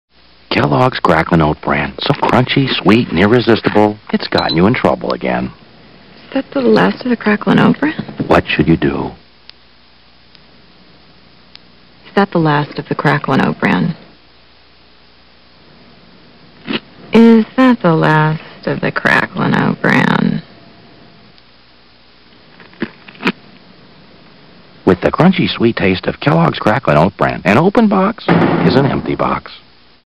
Kellogg's Cracklin' Oat Bran commercial - 1991
Commercial for Kellogg's Cracklin' Oat Bran. Recorded in 1991.